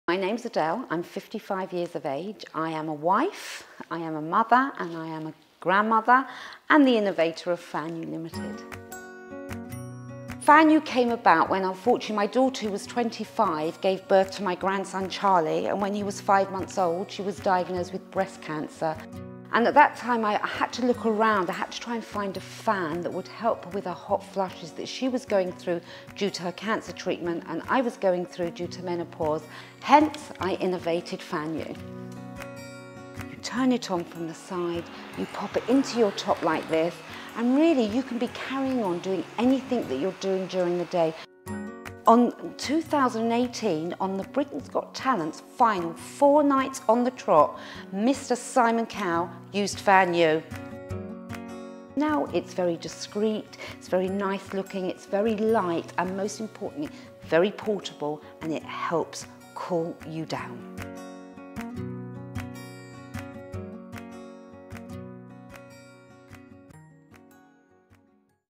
Portable Fans That Will Save Your Next Hot Flush | FanU | Goddiva Marketplace
We're excited to announce the launch of our marketplace, with exquisite, unique brands, like FanU. FanU is a family business set up by Adele and daughter Jodi. Jodi was sadly diagnosed with breast cancer at the age of 25 and suffered with hot flushes as a chemotherapy side effect. Adele was simultaneously going through menopause and struggling herself with hot flushes due to hormonal changes. There were no fans on the market that seemed adequate… they were all bulky or unattractive with short battery life. As a result Adele spent time sourcing a great fan and after some consideration, FanU was born!